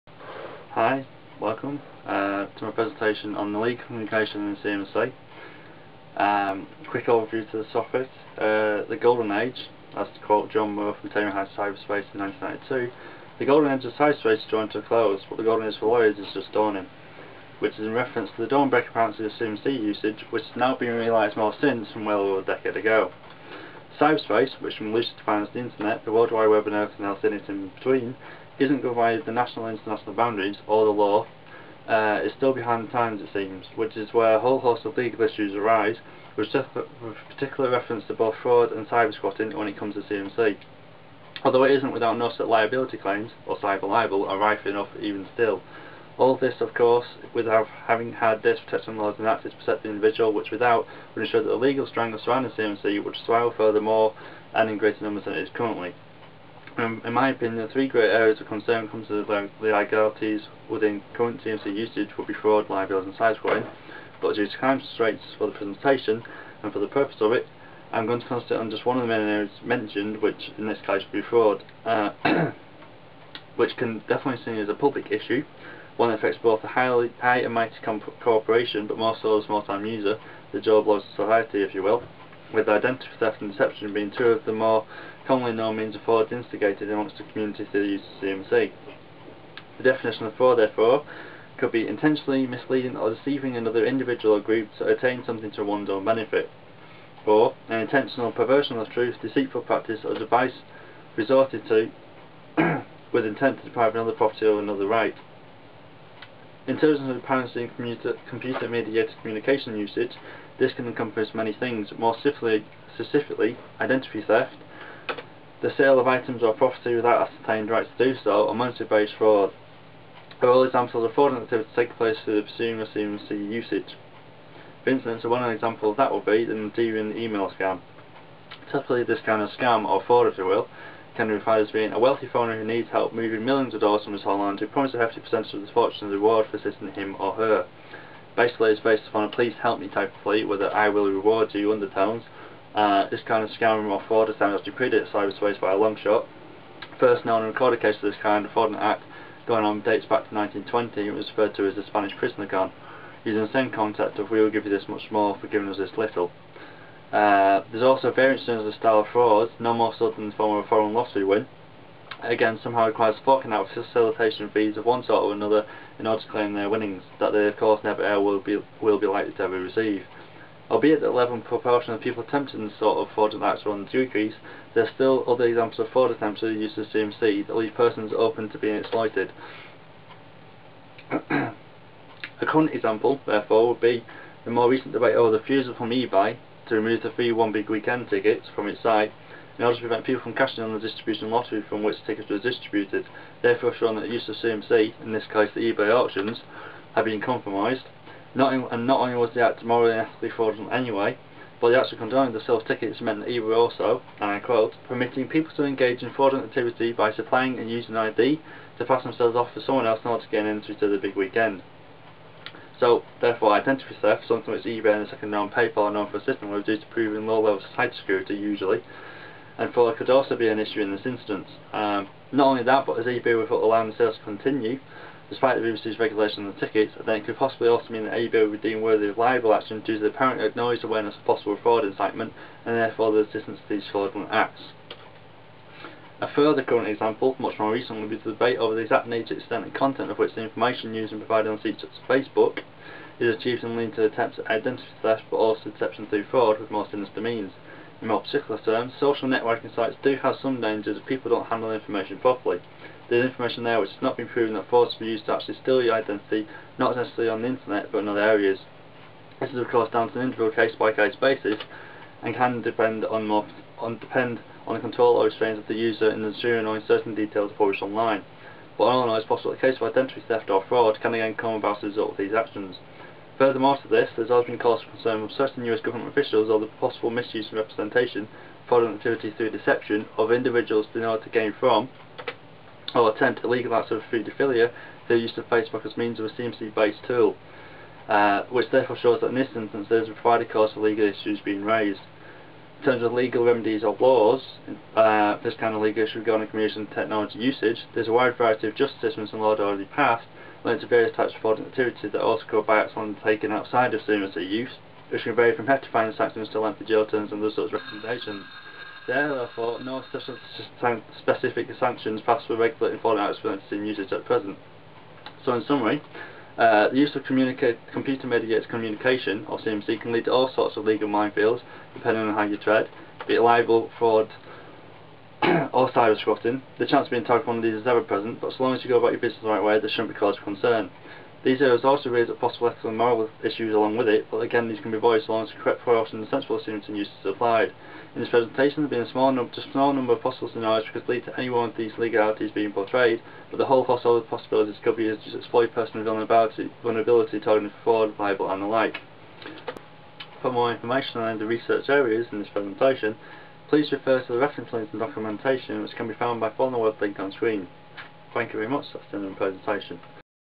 Legal Communication in CMC
The use of Communication Technology for Legal Communication.

UCLan 'Communication  Technology for Social Spaces' Assignment - Presentation Video.